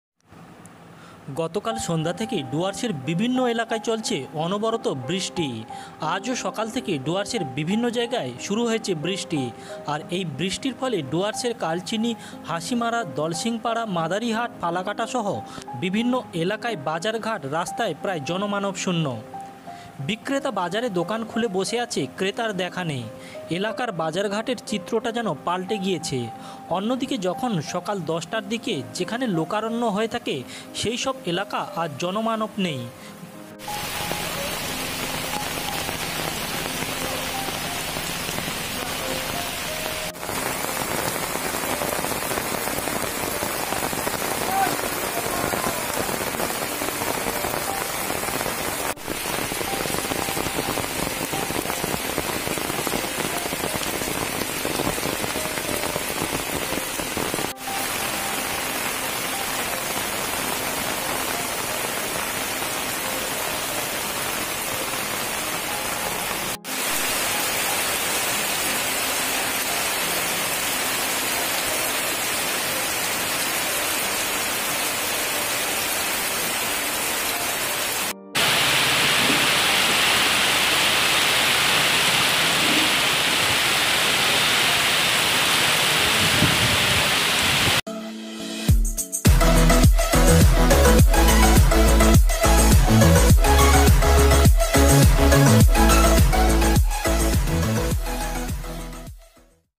ডুয়ার্সের বিভিন্ন এলাকায় চলছে অনবরত বৃষ্টি।
আলিপুরদুয়ার, নিজস্ব সংবাদদাতা:- গতকাল সন্ধ‍্যা থেকে ডুয়ার্সের বিভিন্ন এলাকায় চলছে অনবরত বৃষ্টি আজ ও সকাল থেকে ডুয়ার্সে বিভিন্ন জায়গায় বৃষ্টি। আর এই বৃষ্টি ফলে ডুয়ার্সে কালচিনি , হাসিমারা, দলসিংপাড়া,মাদারিহাট,ফালাকাটা সহ বিভিন্ন এলাকায় বাজার ঘাট রাস্তায় প্রায় জনমানব শূন‍্য ।

বিক্রেতা বাজারে দোকান খুলে বসে আছে ক্রেতার দেখা নেই ।  এলাকার বাজার ঘাটের চিত্র টা যেন পালটে গিয়েছে অন‍্যদিন যখন সকাল দশটার দিকে যেখানে লোকারণ্য হয়ে থাকে সেই সব এলাকায় আজ জনমানব নেই বললেই চলে মানুষ ঘরথেকে বেরোচ্ছে না । যাদের খুব প্রয়োজন তারা শুধু বেরোচ্ছে ।